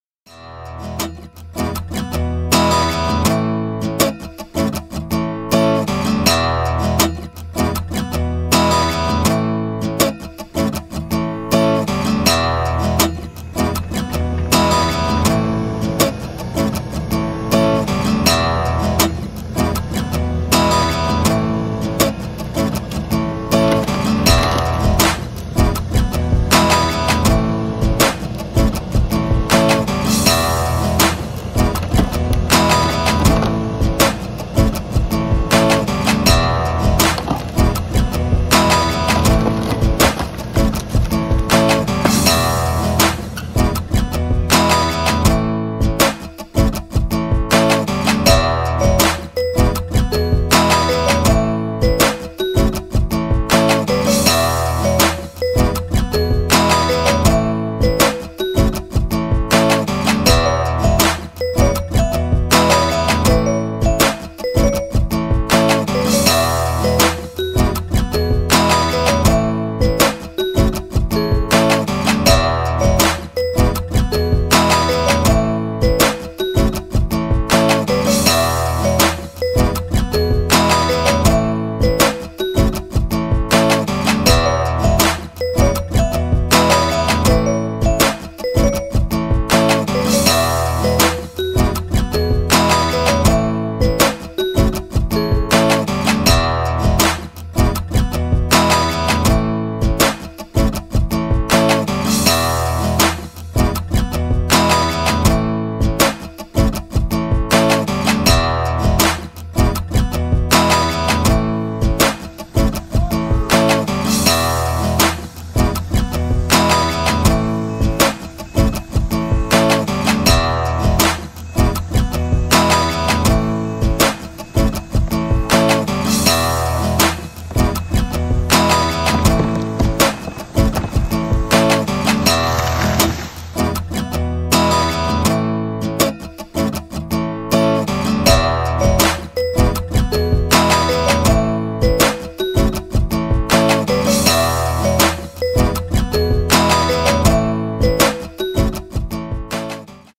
Clearing underbrush with a midi excavator and taking 1 1/4 dead pine trees down
Using a bucket and thumb to take down dead trees and uproot underbrush.